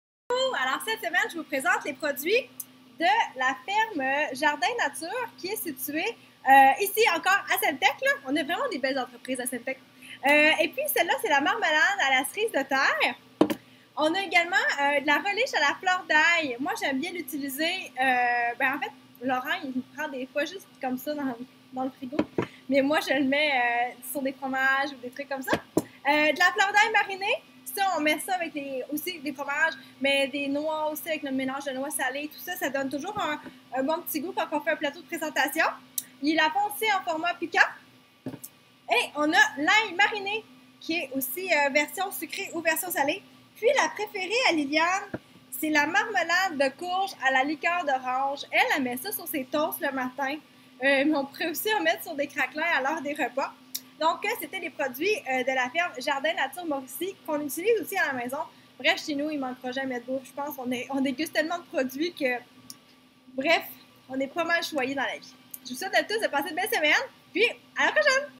Présentation Jardin nature Mauricie
Leur ferme maraîchère biologique se situe à Sainte-Thècle en Mauricie.: Dans leur entreprise familiale, les produits sont cultivés, récoltés et transformés à la main avec beaucoup de cœur.  Soucieux d'offrir des aliments de qualité, beaux et bons et de réduire le plus possible le pourcentage de pertes, les récoltes se font sur une base journalière.

Ils vendent leur ail ainsi que leur produit transformer chez Grano :)